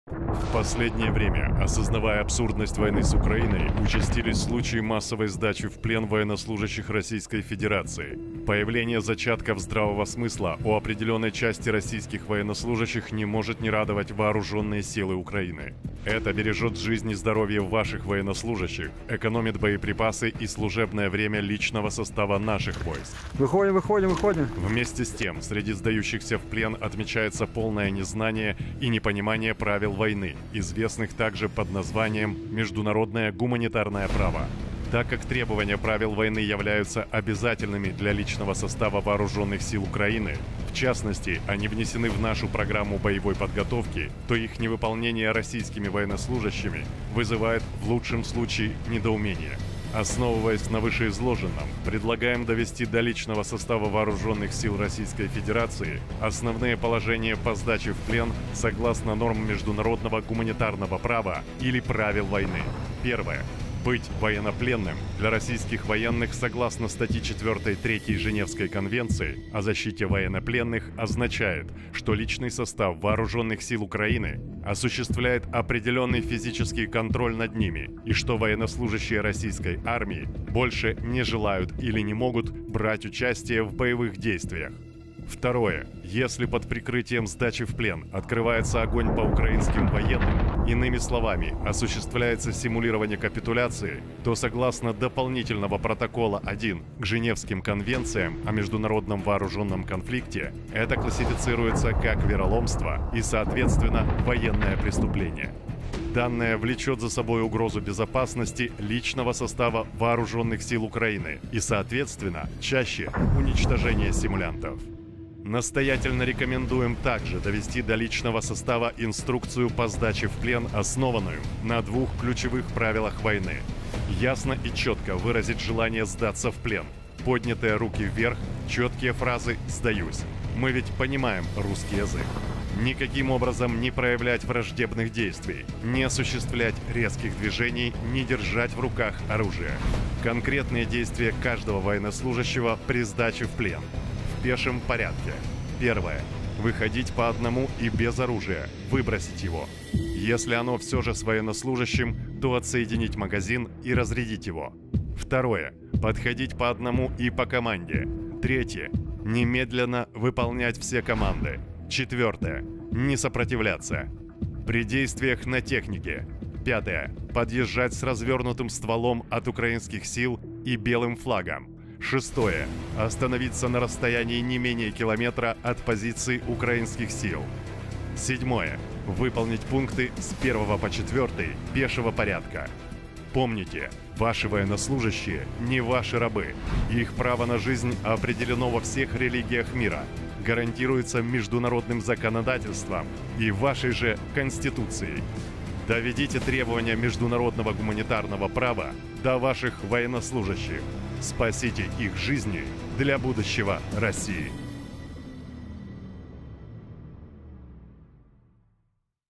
Терміново! Офіційне звернення командування Збройних Сил України до військовослужбовців рф
‼️ ТЕРМІНОВО / СРОЧНО ‼️

Офіційне звернення командування Збройних Сил України до військовослужбовців рф / Официальное обращение командования Вооруженных Сил Украины к военнослужащим рф

Командири та начальники російської окупаційної армії. Добровільна здача в полон це шанс для вас і ваших підлеглих залишитися живими. Багато хто цим шансом вже скористався. Україна дотримується міжнародного права, суворо виконуючи положення Женевської конвенції про поводження з військовополоненими, гарантує їм право на життя та гідність.

Щоб ми могли гарантувати вашу безпеку під час здачі в полон, змусьте вивчити та проконтролюйте рівень засвоєння наступної покрокової інструкції.

Командири та начальники російської окупаційної армії! Ваші підлеглі – не раби. Їхнє право на життя визначене у всіх релігіях світу, гарантоване міжнародним законодавством та вашою ж Конституцією. Врятуйте їхнє життя для майбутнього росії.

Командиры и начальники российской оккупационной армии. Добровольная сдача в плен это шанс для вас и ваших подчиненных остаться в живых. Многие этим шансом уже воспользовались. Украина соблюдает международное право, строго выполняя положения Женевской конвенции об обращении с военнопленными, гарантирует им право на жизнь и достоинство.

Чтобы мы могли гарантировать вашу безопасность во время сдачи в плен, заставьте выучить и проконтролируйте степень усвоения следующей пошаговой инструкции.

Командиры и начальники российской оккупационной армии! Ваши подчиненные – не рабы. Их право на жизнь определено во всех религиях мира, гарантировано международным законодательством и вашей же Конституцией. Спасите их жизни для будущего россии.